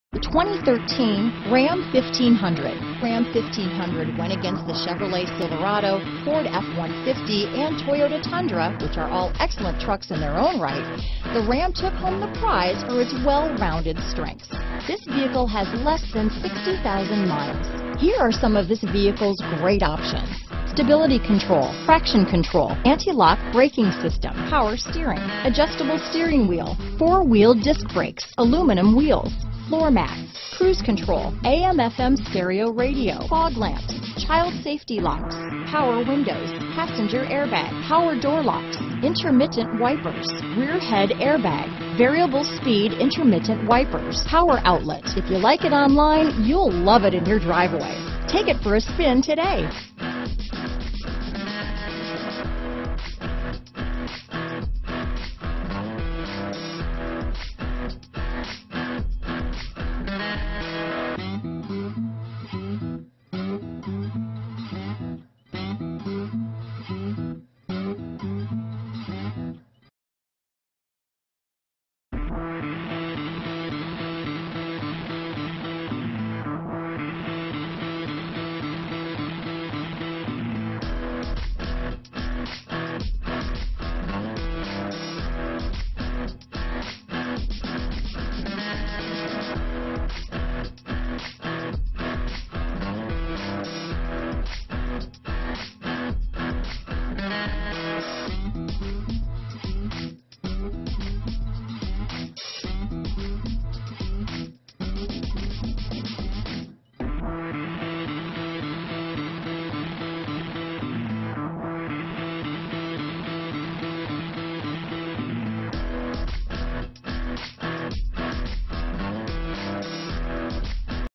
2013 Ram 1500 Phoenix, Scottsdale, Tempe, Peoria, Mesa, AZ P10253A
Bright Silver Metallic Used 2013 Ram 1500 available in Phoenix, Arizona at Airpark Chrysler Dodge Jeep. Servicing the Scottsdale, Tempe, Peoria, Mesa, AZ area.

Airpark CDJ
7801 E Frank Lloyd Wright Blvd

New Price! Silver 2013 Ram 1500 HEMI 5.7L V8 Multi Displacement VVT AutomaticOdometer is 7241 miles below market average! Awards:* NACTOY North American Truck of the Year